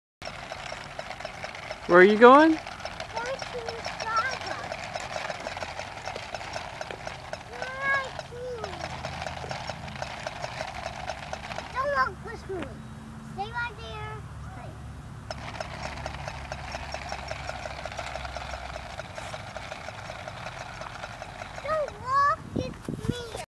Going to Starbucks
Etta taking her dog to Starbucks